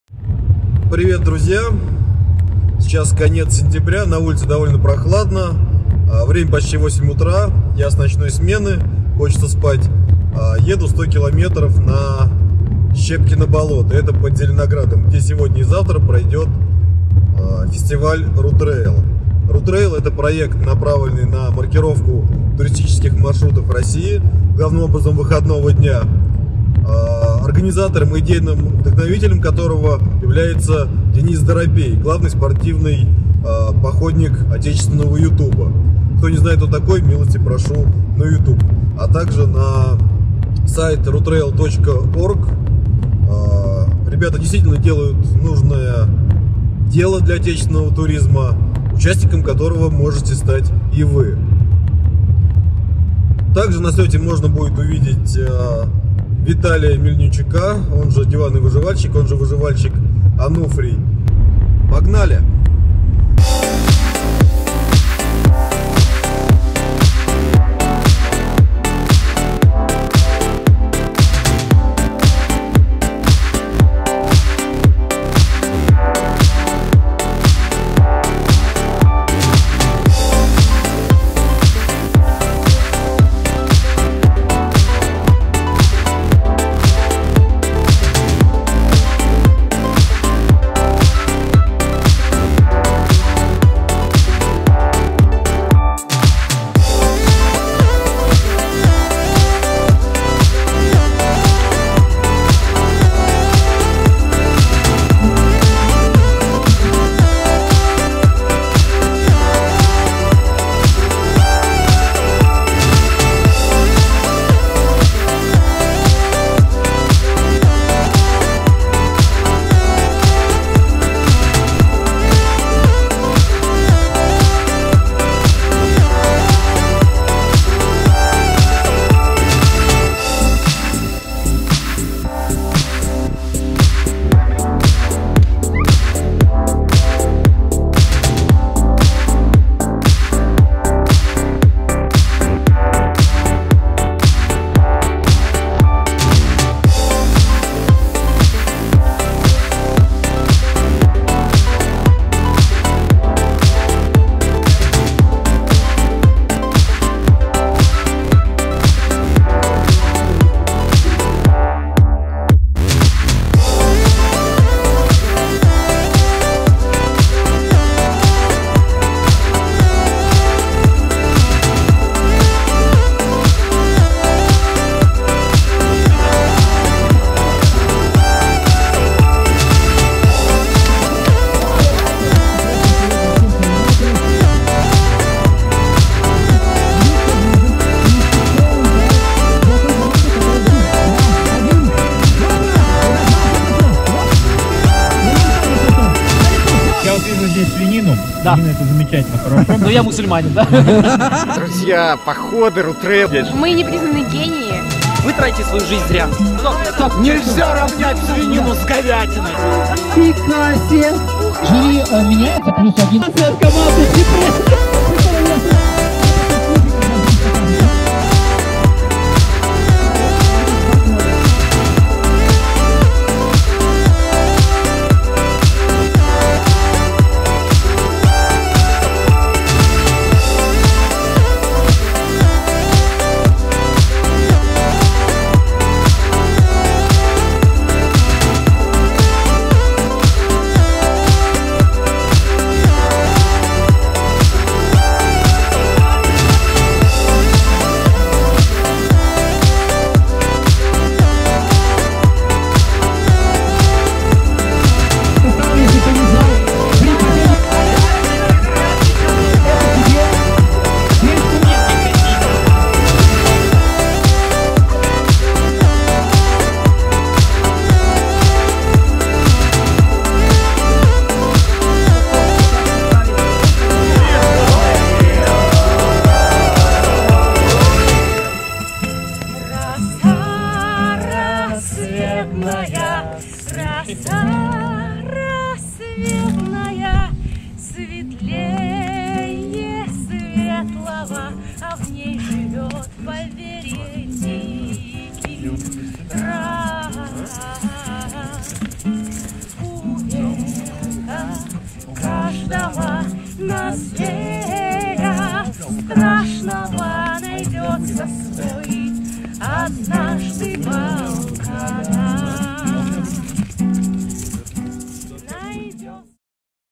Фестиваль RuTrail на Щепкином болоте. Осень 2018
Краткая версия выходных конца сентября, во время которых на Щепкином болоте прошел фестиваль проекта RuTrail.org

Главные лица и организаторы фестиваля:

А также еще множество приятных и позитивных людей.